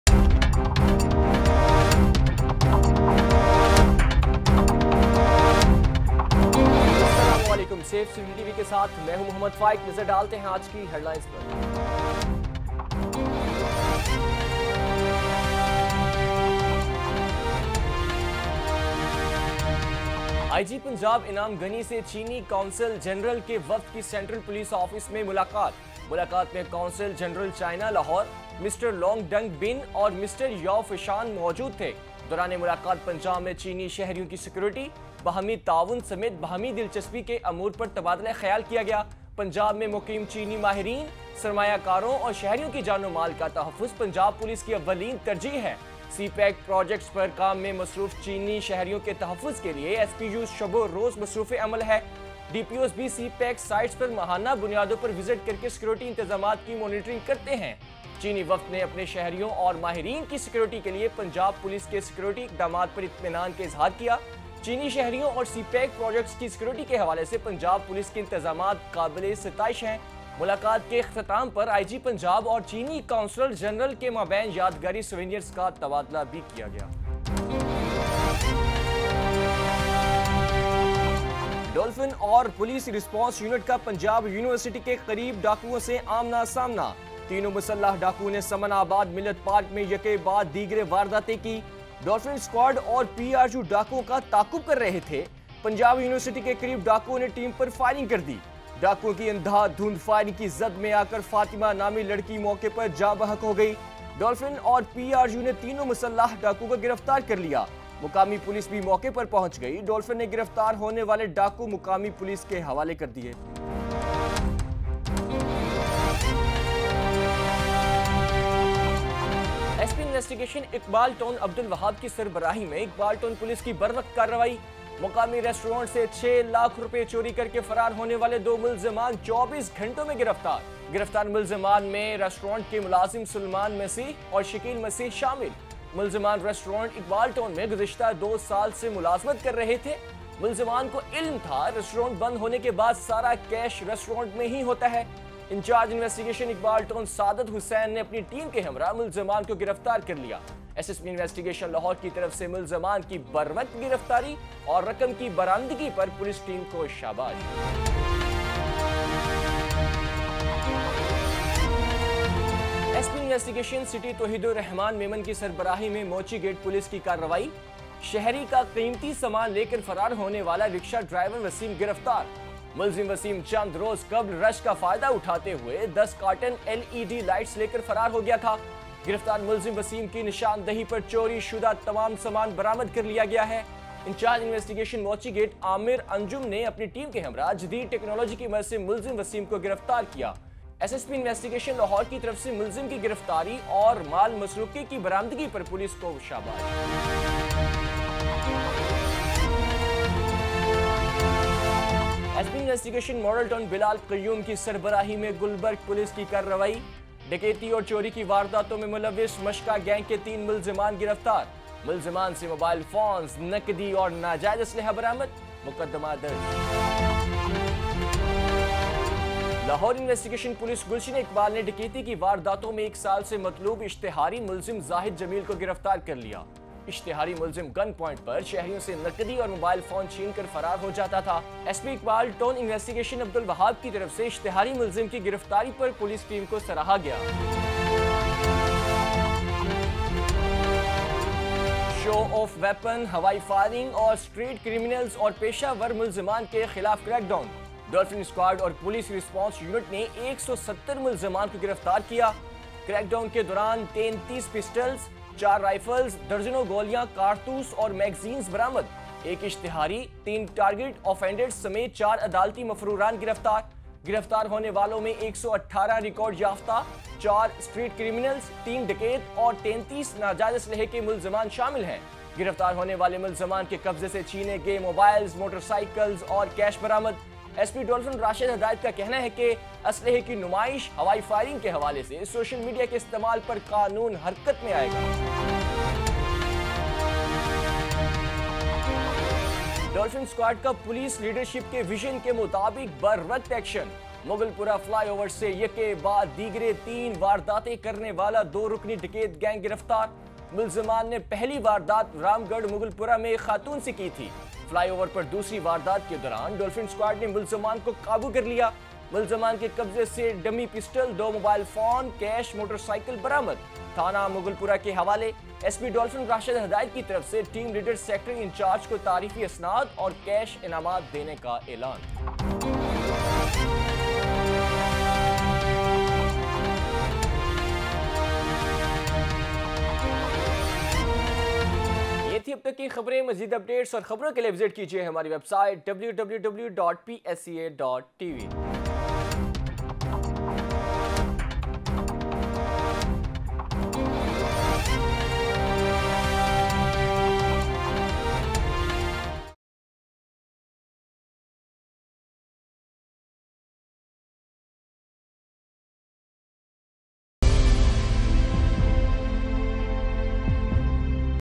Dolphin Police squad fights robbers near Punjab University | Safe City News | 22th October, 2020
Punjab Police PRU and Dolphin police squad fight with robbers near Punjab University. Dolphin police force caught the robbers but a women has died in exchange of firing between Police and robbers.  PSCA and Punjab Police News Updates